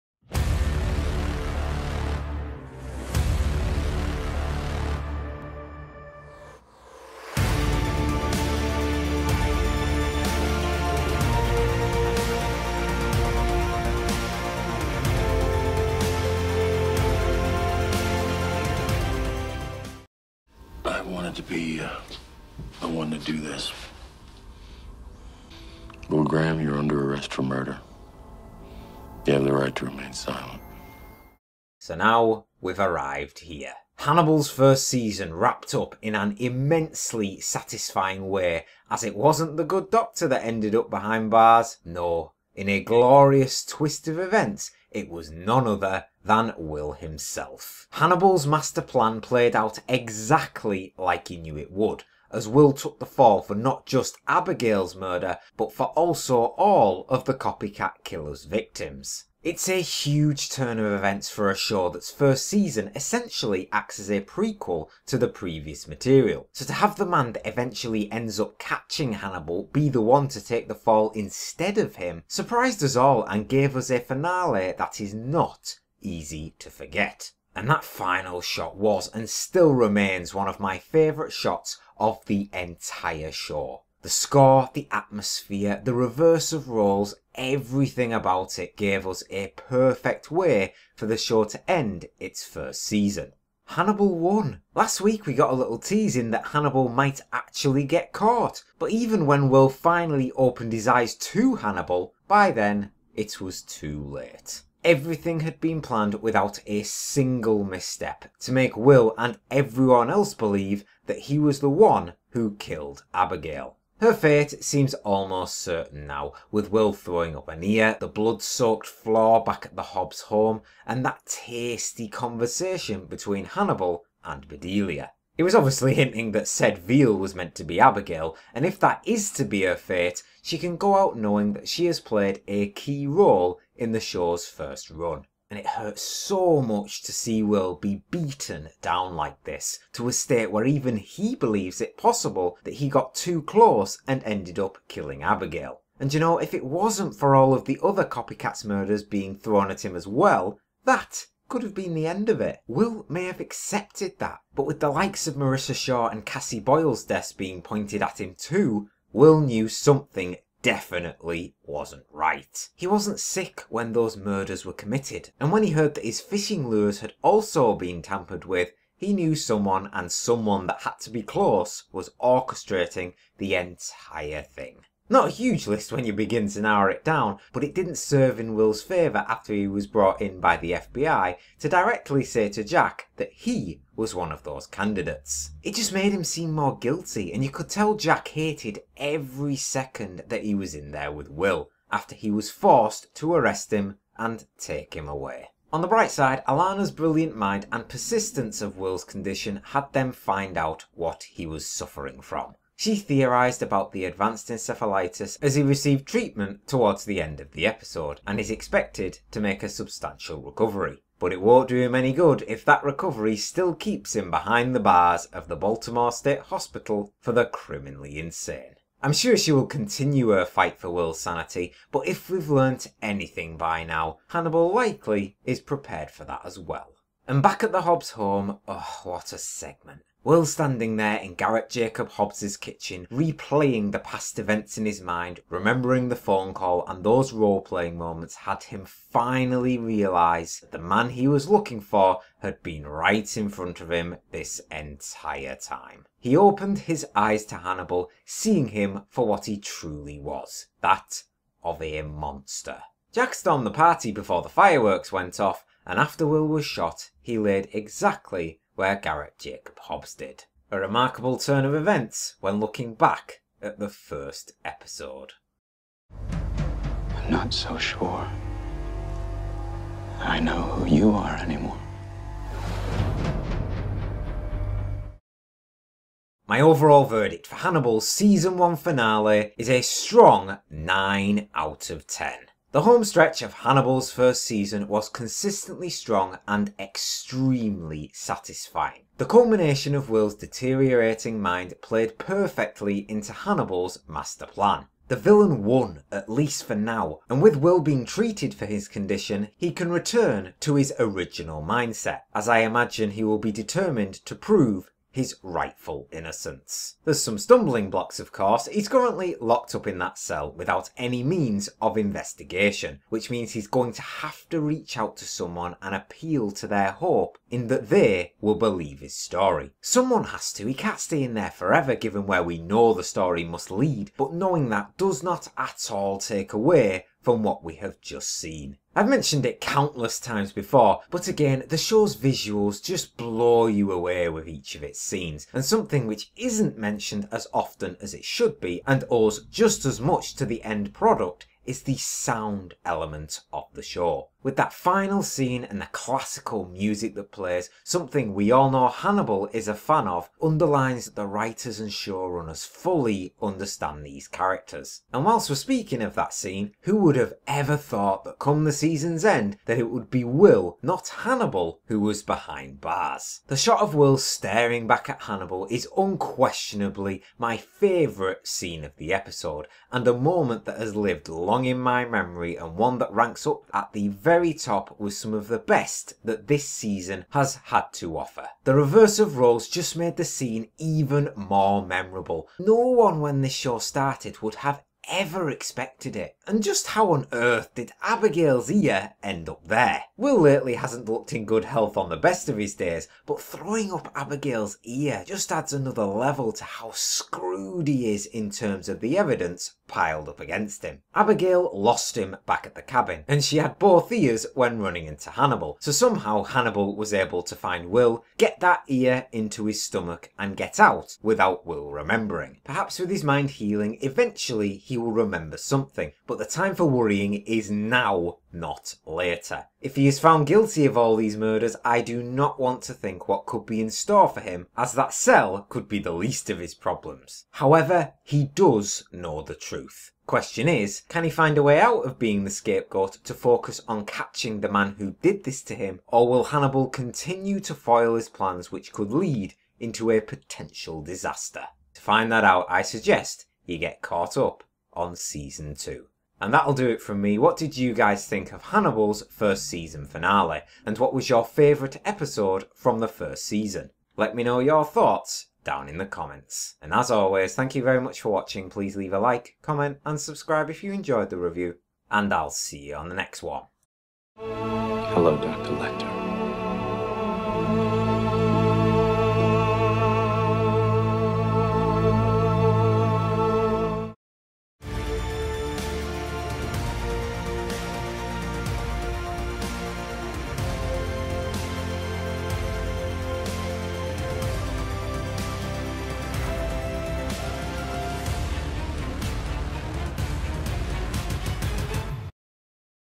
Hannibal Season 1 Episode 13 Review - 'SAVOUREUX'
Will is taken into custody for the murder of Abigail Hobbs, as Hannibal’s master plan plays out to perfection.

Hannibal Season 1 Episode 13 ‘Savoureux’ Review.